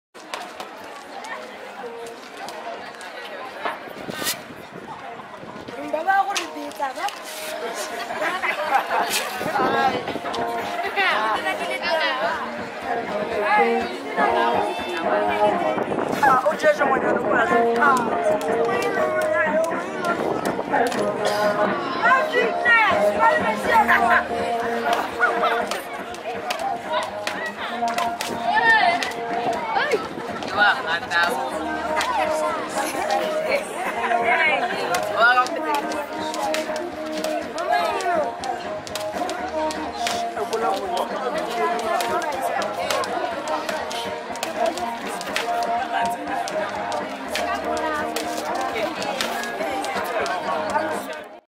un tour du monde en 80 pays en voiture 4x4 afrique botswana un collège qui organise sa fête annuelle
un collège qui organise sa fête annuelle, démonstration de boxe.